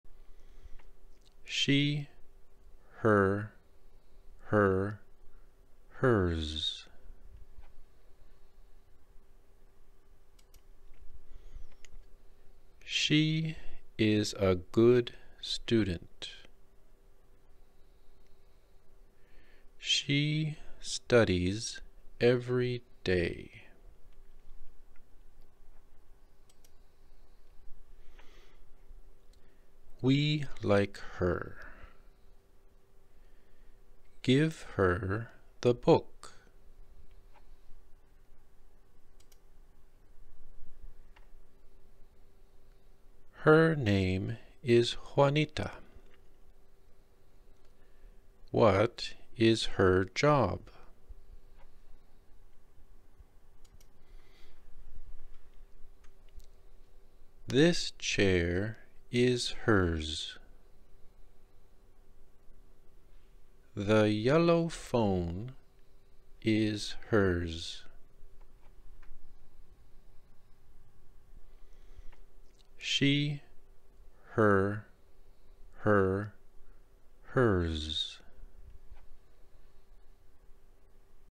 Pronoun Chart She
subject pronoun: she
object pronoun: her
possessive adjective: her
possessive pronoun: hers